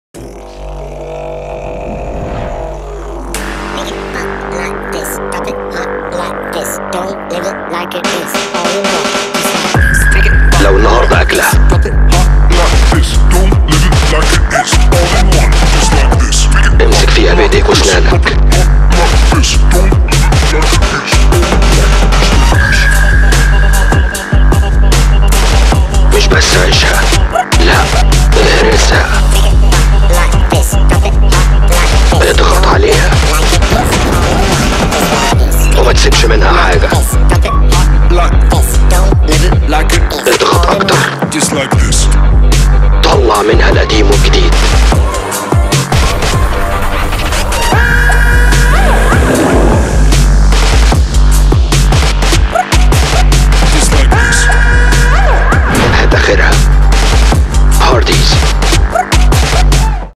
Hardee's | VO Recording | Egyptian Male Talent
Project:
  - Hardee's
Client:
  - Big Kahuna Films

Job:
  - VO Recording
  - Sound Editing

Talent:
  - Male
Language:
  - Egyptian